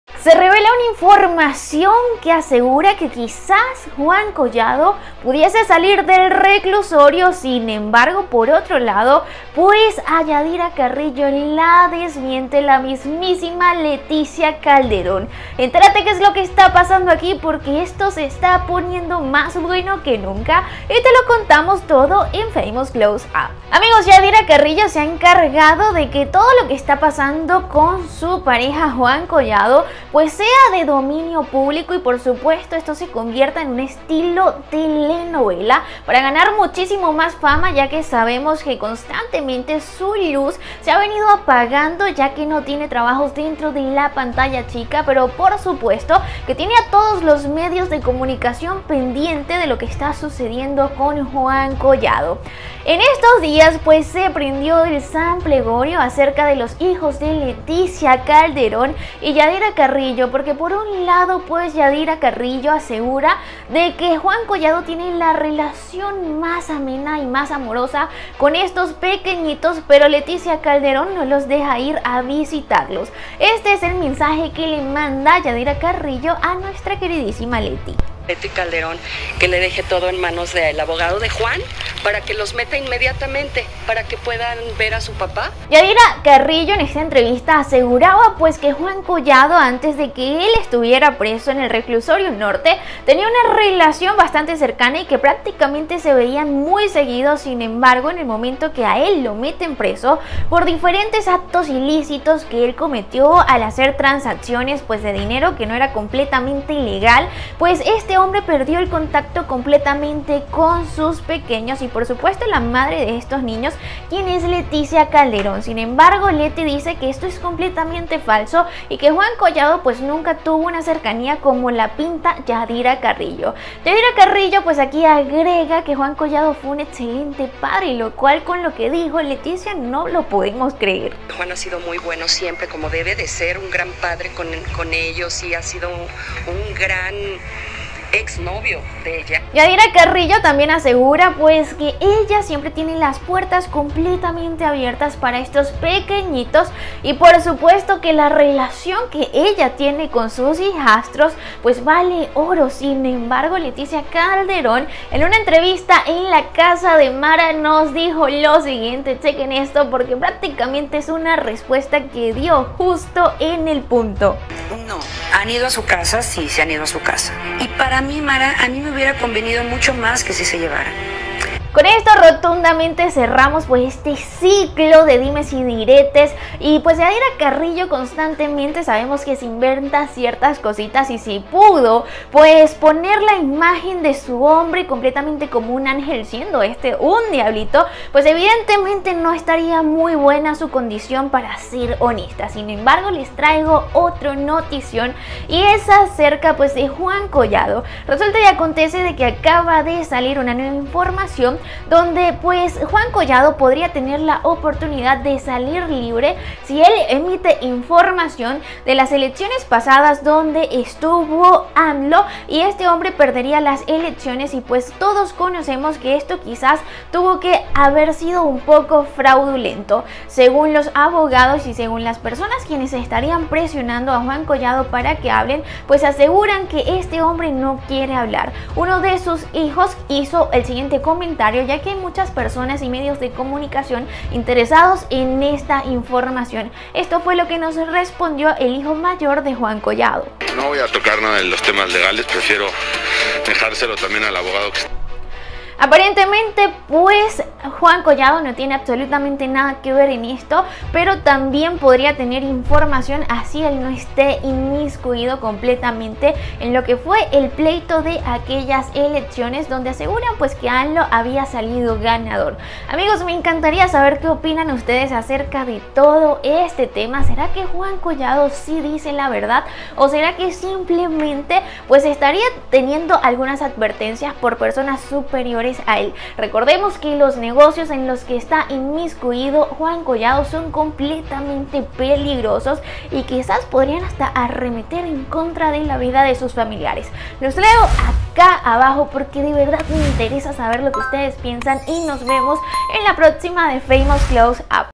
🚨😱 ¡ 𝗨́𝗹𝘁𝗶𝗺𝗮 𝗛𝗼𝗿𝗮 Leticia Calderón 𝗘𝘅𝗵𝗶𝗯𝗲 𝗟𝗮 𝗛𝗶𝗽𝗼𝗰𝗿𝗲𝘀𝗶𝗮 de Yadhira Carrillo ! ⛔😡
Se revela que Juan Collado pudiera salir del reclusorio, mientras Yadhira Carrillo es desmentida por Leticia Calderón acerca de la relación con sus hijos